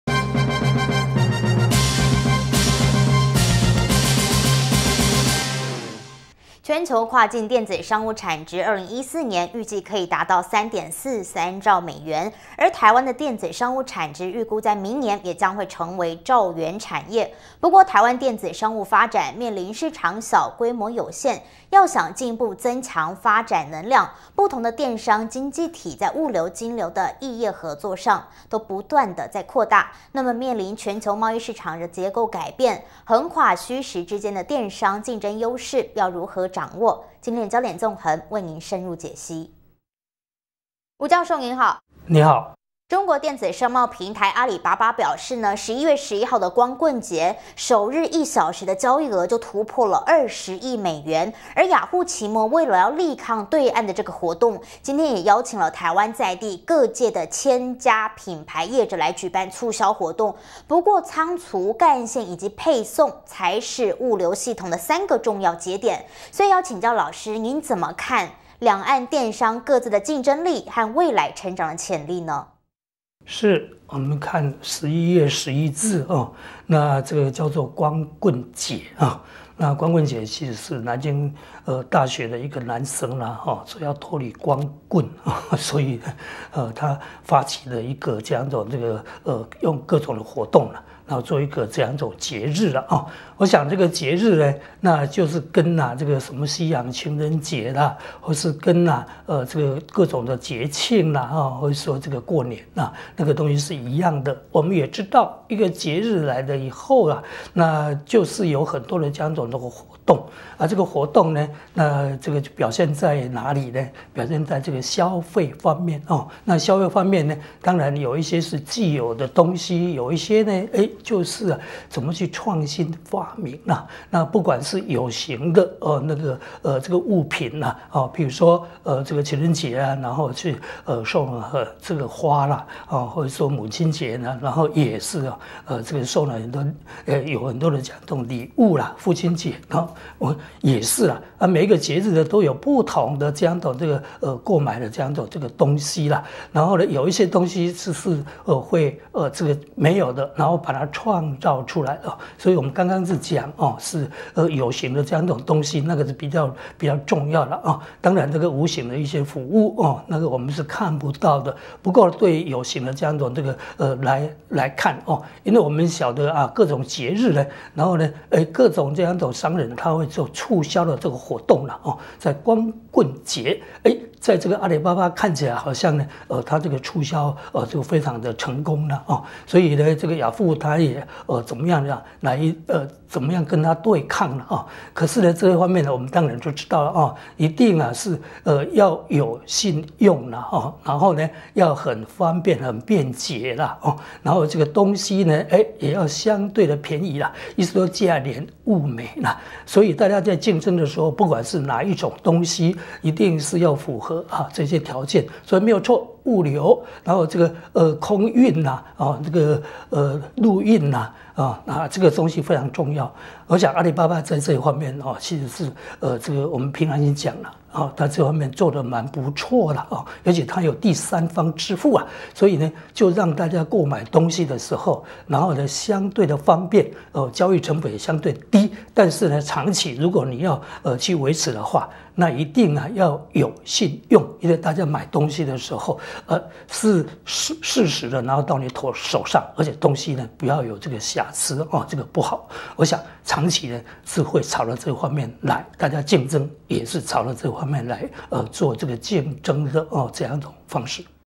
【新唐人/NTD】虛實之間的競爭力 電子商務新兆元產業｜財經縱橫｜光棍節｜阿里巴巴｜Yahoo奇摩
全球跨境電子商務產值，2014年，預計可達到3.43兆美元。而台灣的電子商務產值，預估在明年，也將成為兆元產業。不過，台灣電子商務發展，面臨市場小、規模有限，要想進一步增強發展能量，不同的電商經濟體在物流、金流的異業合作上，都不斷再擴大，面臨全球貿易市場的結構改變，橫跨虛實之間的電商競爭優勢如何掌握？今天的焦點縱橫，為您深入解析。